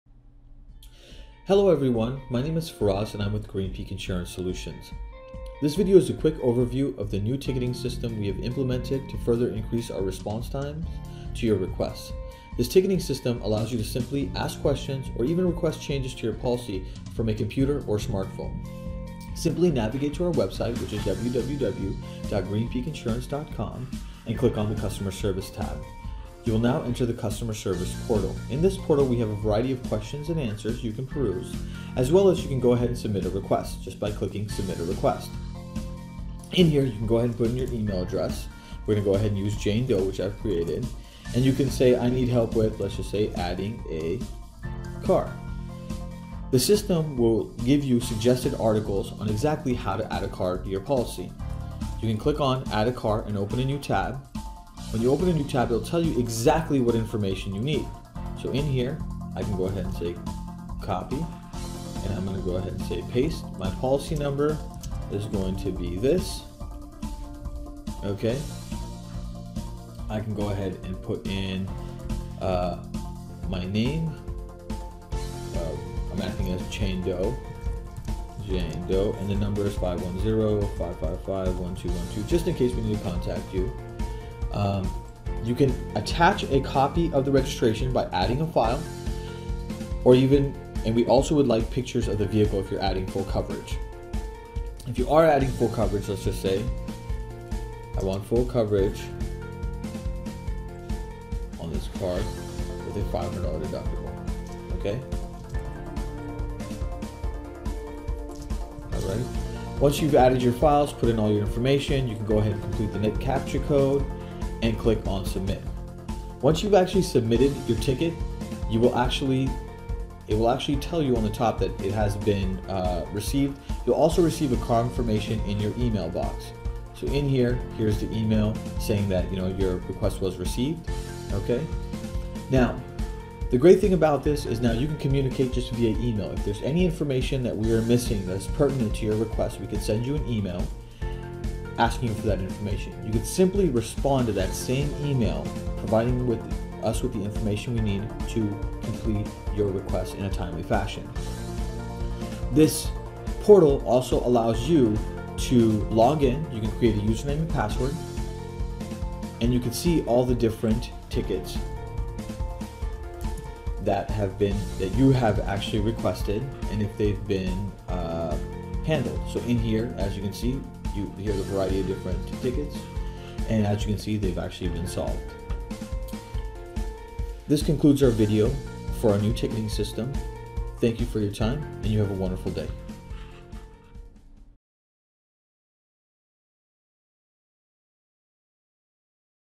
Customer Service Request Ticketing System
Overview on how to use the customer service portal to submit and track request.